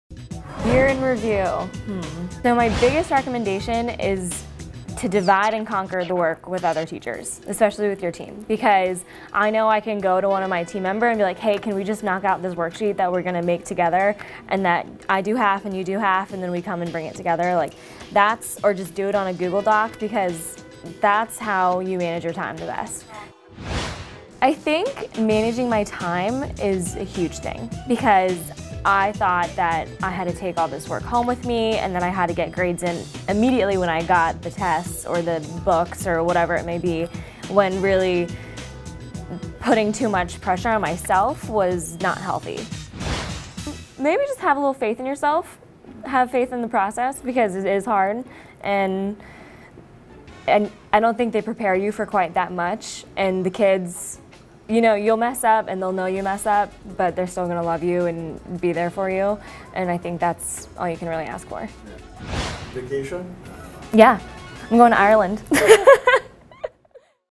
Ellie's Tips for First Year Teacher Success
Ellie joined FCPS in the 2016-17 school year as a teacher at Stuart High School. We spent the year checking in with her at orientation, on the first day and in the middle of the school year. Now, as the year draws to a close, Ellie shares her tips for next year's first year teachers.